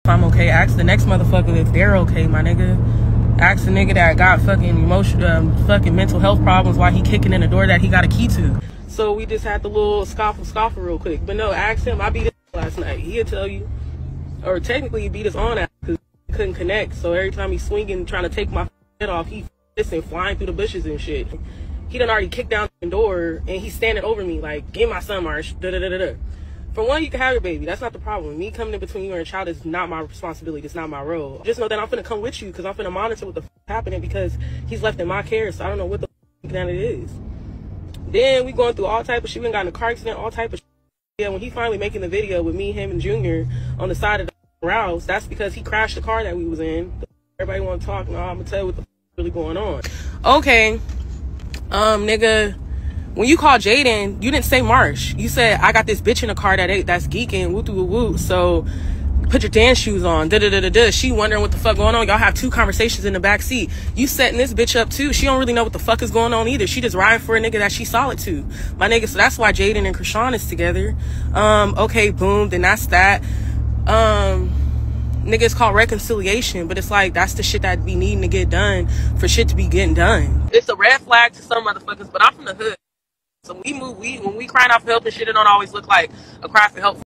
Blueface Gets Into A Car Accident With Baby Jr And Marsh
This is a video of Chrisean rock bff marsh telling her side of the story and also saying blueface got into a car accident with baby jr.